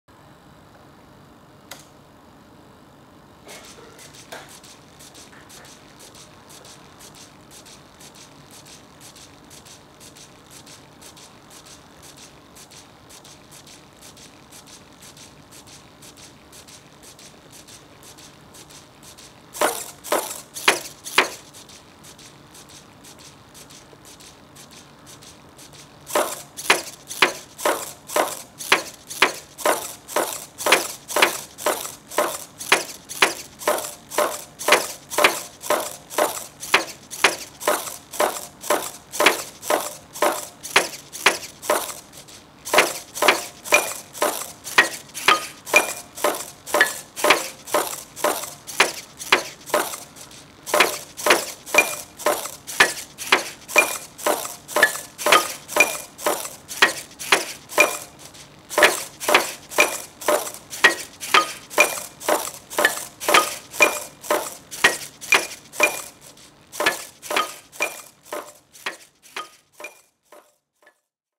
Robot Orchestra
The latest version of NUSTEM's Robot Orchestra: An Adafruit Trellis UNTZtrument acts as a control surface for a Python script on a Raspberry Pi, commanding Wemos D1 mini boards over MQTT.

Full write-up coming soon.